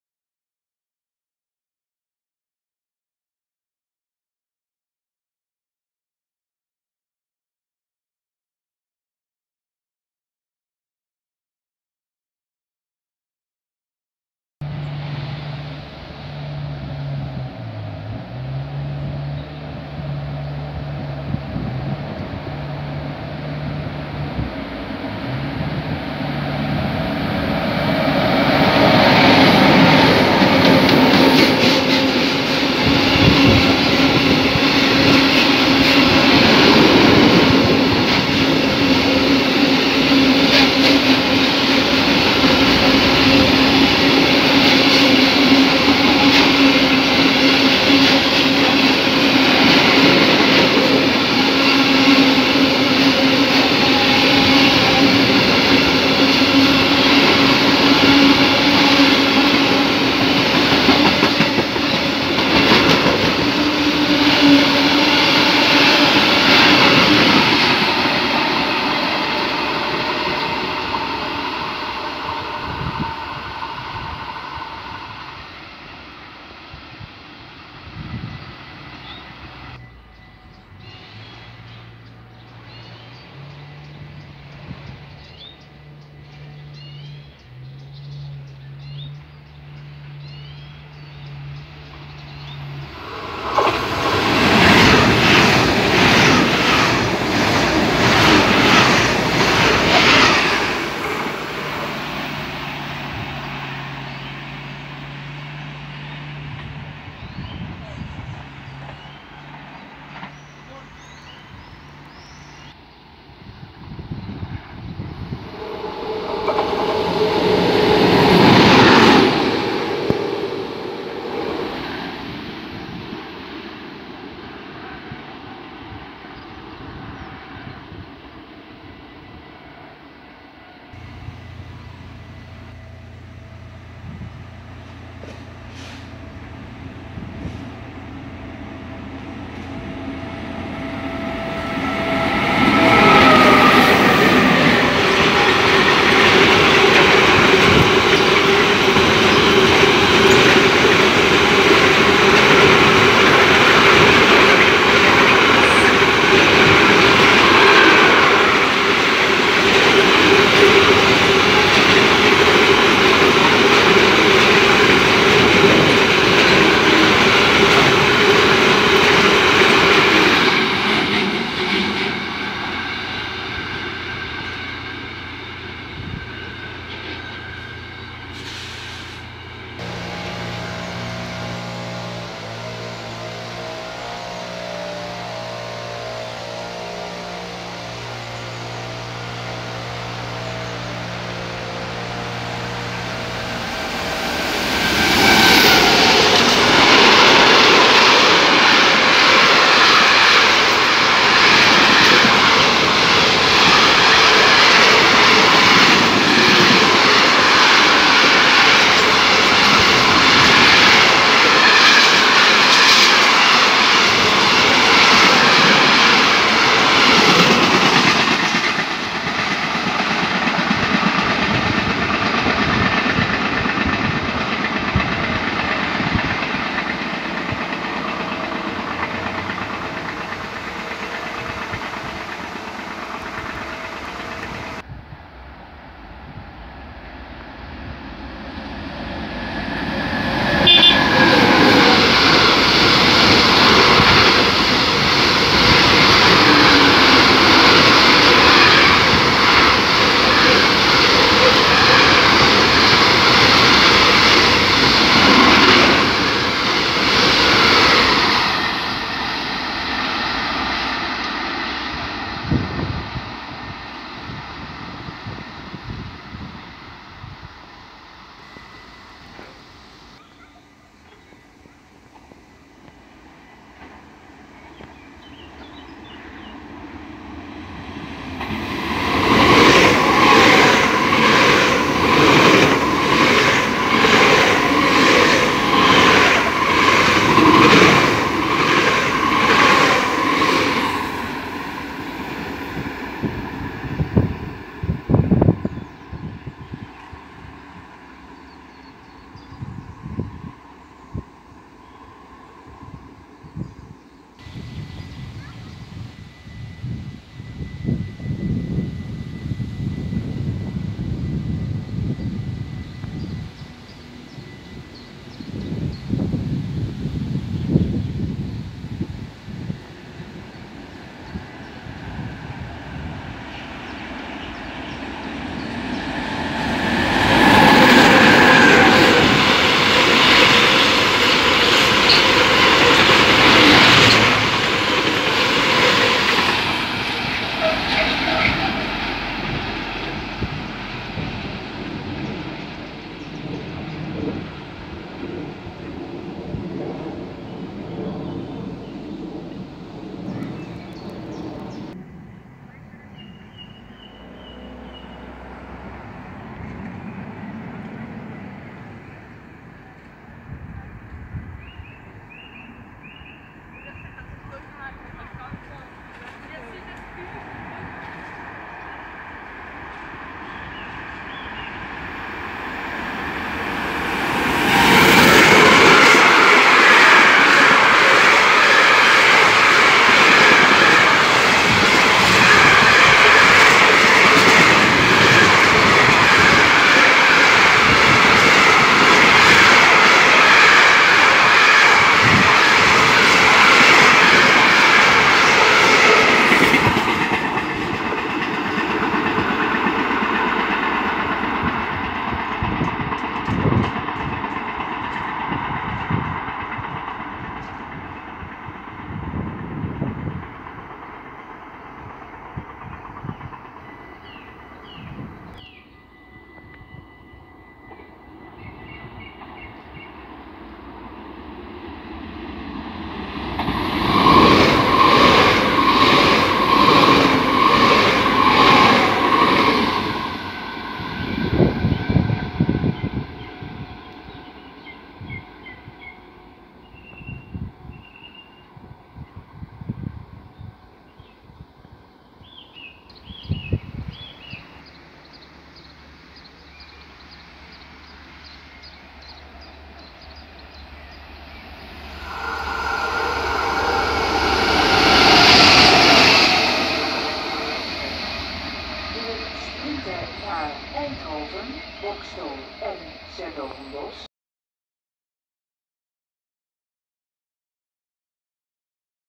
Treinen in Helmond 't Hout - 26 Mei 2018
Met: DB Cargo E 189-823 (Wit) – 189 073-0 met een kolentrein richting Boxtel.
DB ICE 3 4602 als Internationale trein richting Venlo.
RTB Cargo V155 (g1206) als losse loc naar Blerick.
DB Cargo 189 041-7 – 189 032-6 met een ertstrein richting Boxtel.
RTB Cargo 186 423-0 (PCC Intermodal/Rurtalbahn Cargo) met een PCC-shuttle naar de Kijfhoek.
Captrain E 189-091 met een ERR kolentrein richting de Kijfhoek (tyfoon).
Ns VIRM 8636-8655 als Intercity naar Schiphol Airport.
DB Cargo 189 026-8 – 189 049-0 met een beladen Kalktrein richting ’s-Hertogenbosch.
RTB Cargo 186 107 met een Blerick-shuttle richting Tilburg (8 Mei 2018).
Ns VIRM 8610 als Intercity naar Schiphol Airport (8 Mei 2018).
RRF 561-03 als losse loc richting Eindhoven (8 Mei 2018).
Bekijk ook eens de RailCam in Mierlo-Hout (Helmond 't Hout).

Voor Foto's van treinen:

Bedankt voor het kijken.
Zaterdag 26 Mei 2018.